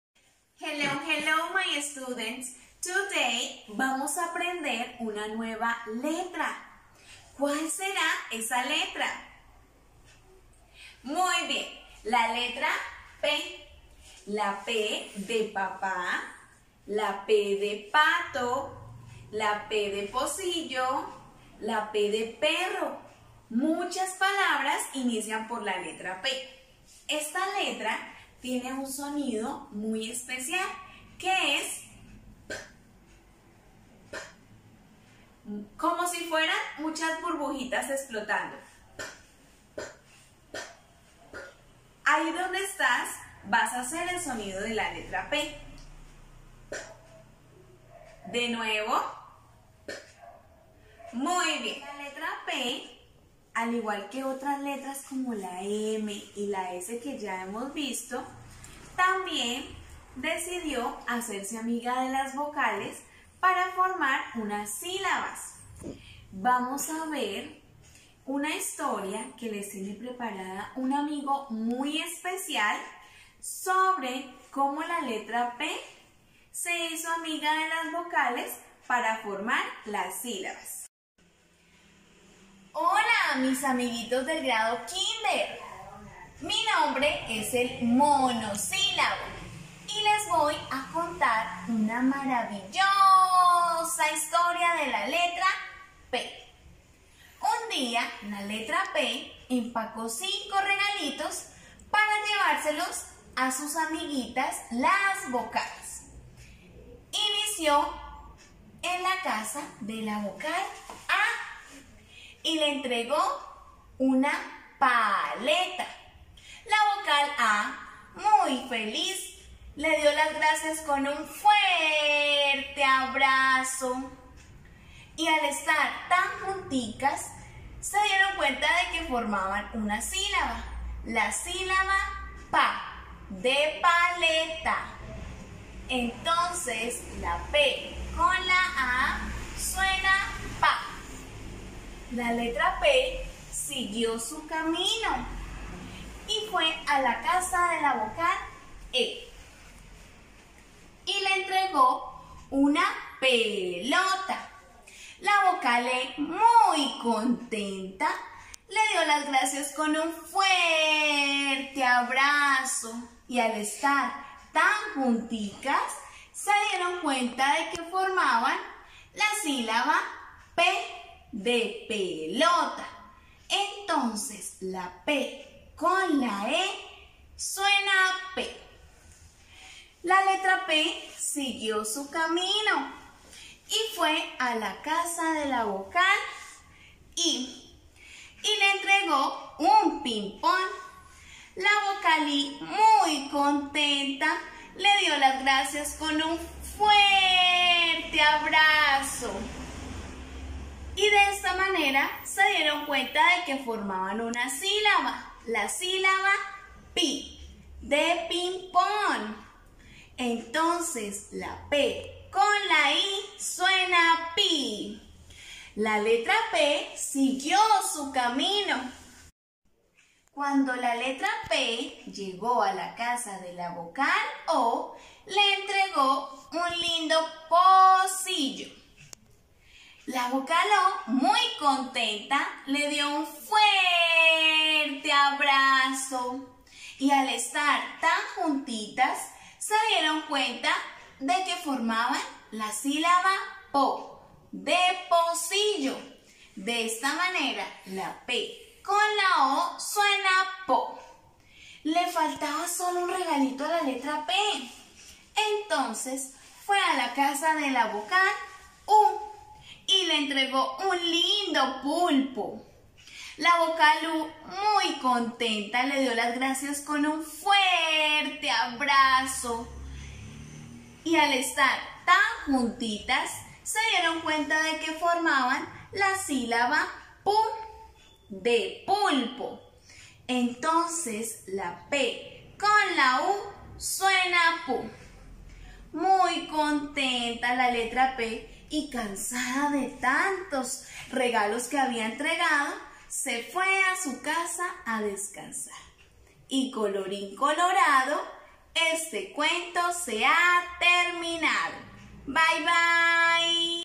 La Letra P
Conoce el cuento de la letra P con sus amigas las vocales y las sílabas.